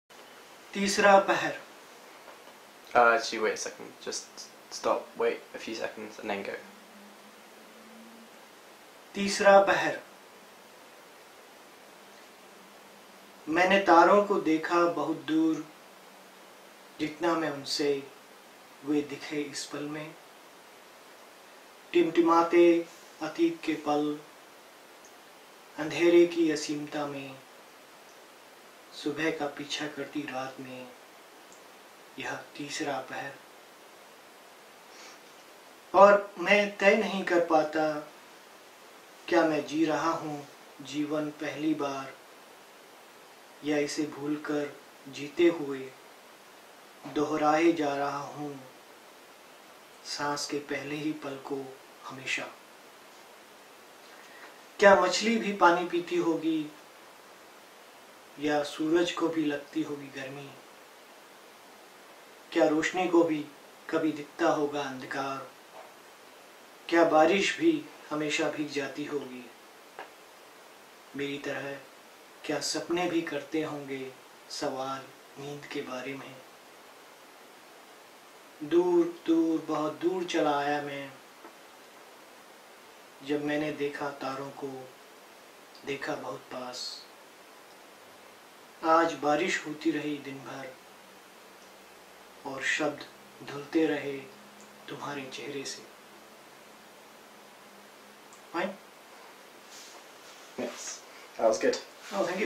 Mohan Rana reading a poem : तीसरा पहर [Teesra Pahar] After Midnight
तीसरा पहर [Teesra Pahar/ After Midnight] by Mohan Rana.
To read English translation of this poem by Bernard O'Donoghue and Lucy Rosenstein please

© Mohan Rana  मोहन राणा